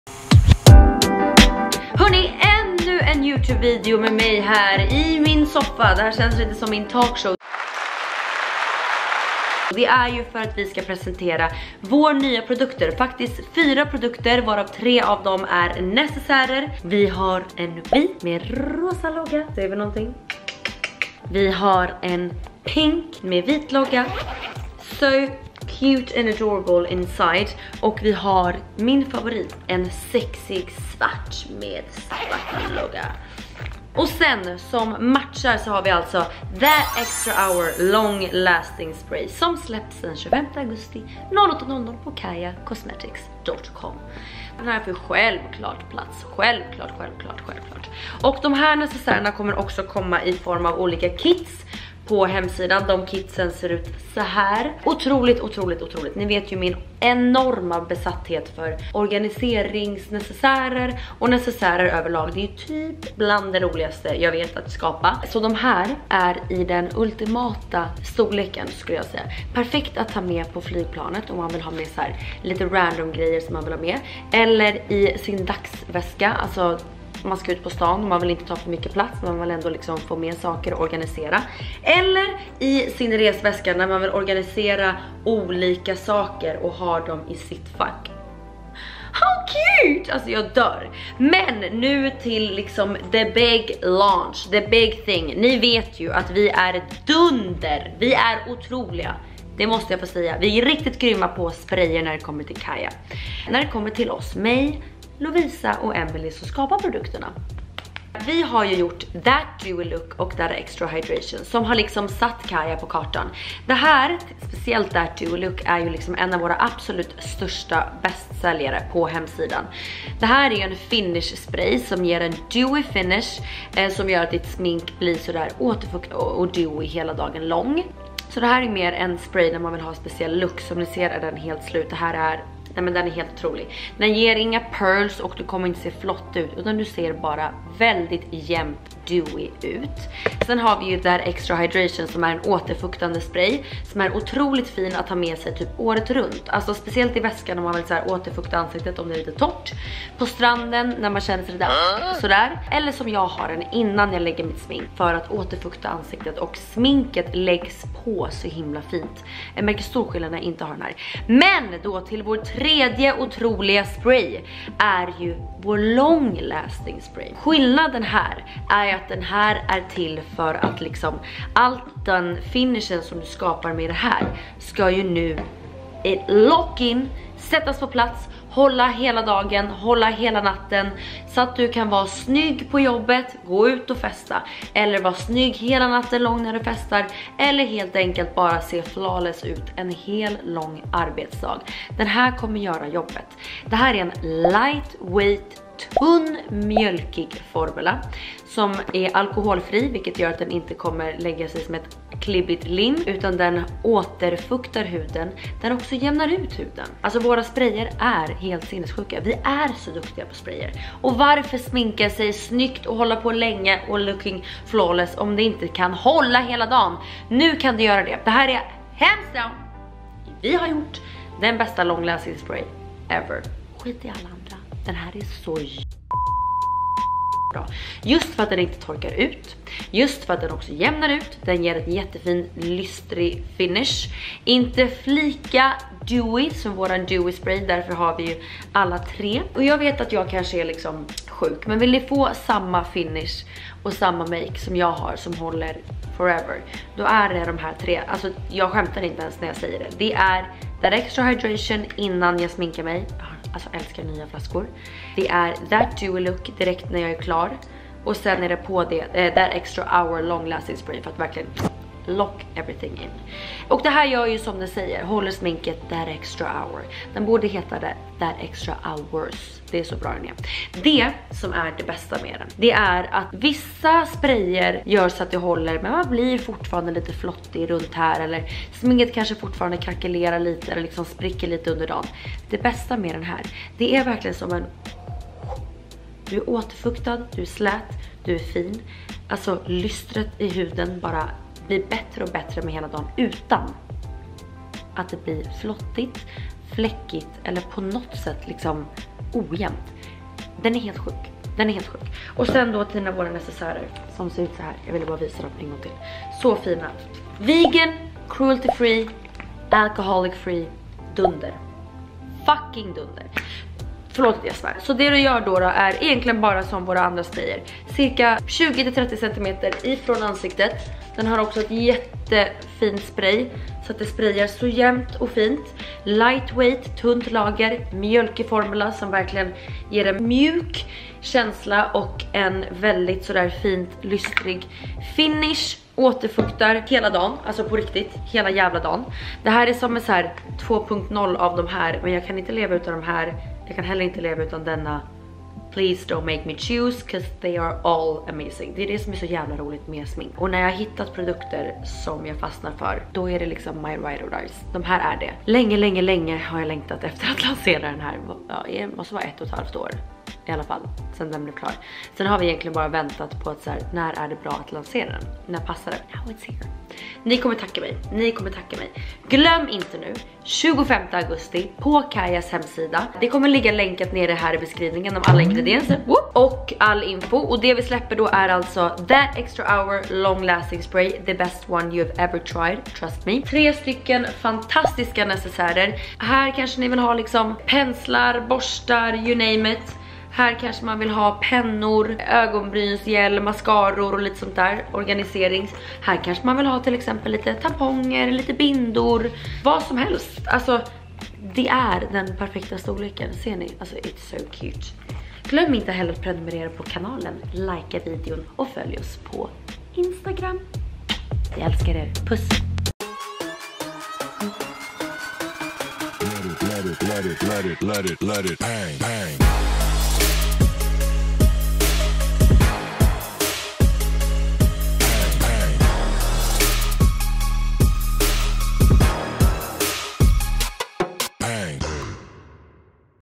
EVERYTHING YOU NEED TO KNOW ABOUT THAT EXTRA HOUR [Swedish]
[Video in Swedish]
Grab a seat and spend some quality time with Bianca and our new setting spray and organizers that will make you stay flawless from day to night! She'll tell you everything you need to know for tomorrow's launch.